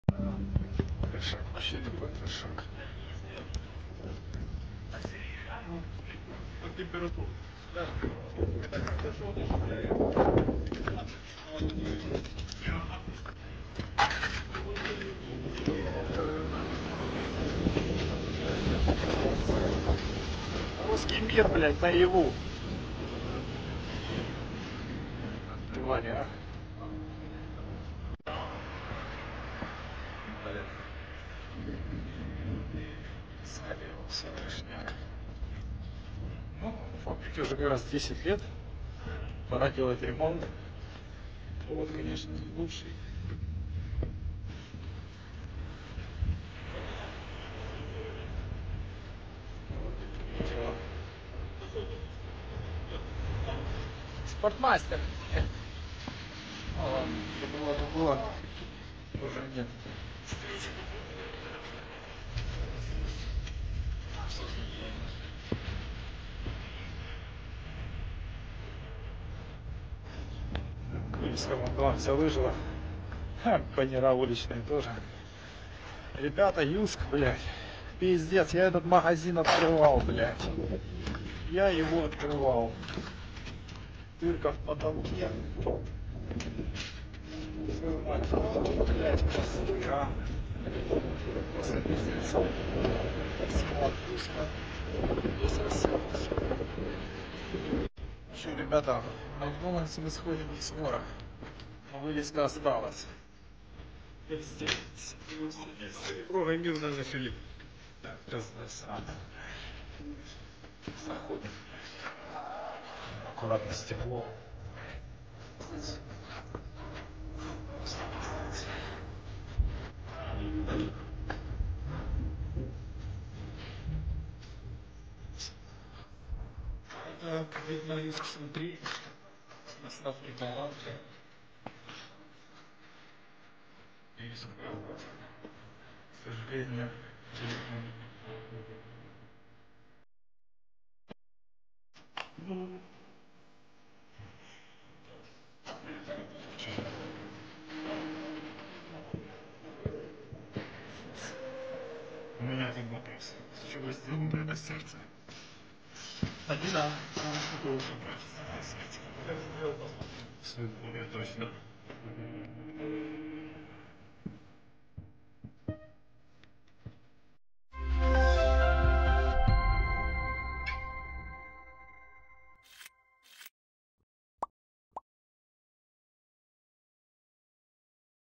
Так выглядит после обстрелов крупнейший на Херсонщине ТРЦ
UA - Місцеві показали, на що перетворився найбільший торгово-розважальний центр у Херсонській області – ТРЦ Fabrika після обстрілу російськими окупантами. Як відомо, наразі Херсон тимчасово окупований.

RU - Местные показали, во что превратился крупнейший торгово-развлекательный центр в Херсонской области – ТРЦ Fabrika после обстрела российскими оккупантами. Как известно, на данный момент Херсон временно оккупирован.